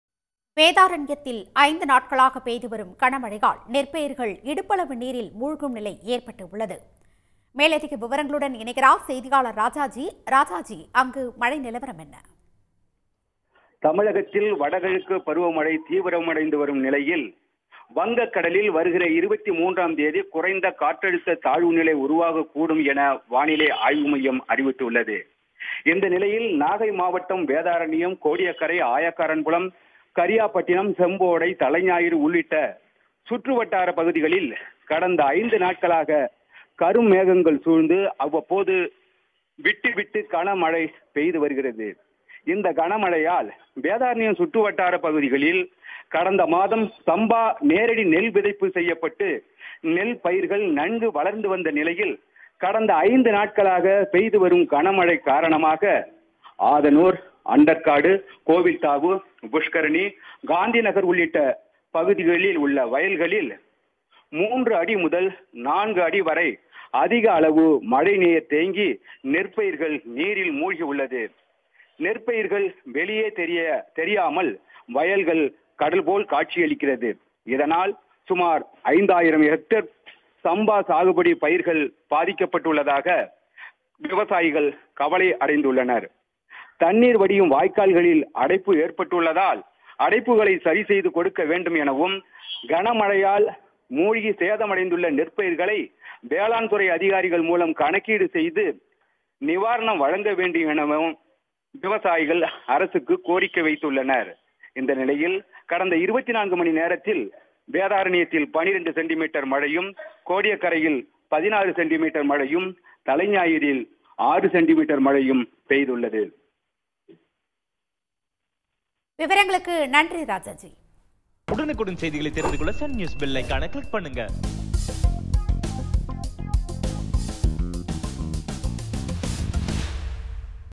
Vedaranyam Rain | Heavy rains continue | 5 Days | Rice crops | Waist-deep water | Farmers worry
வேதாரண்யத்தில் 5 நாட்களாக பெய்துவரும் தொடர் கனமழை

இடுப்பளவு நீரில் மூழ்கிய நெற்பயிர்கள் -விவசாயிகள் கடும் வேதனை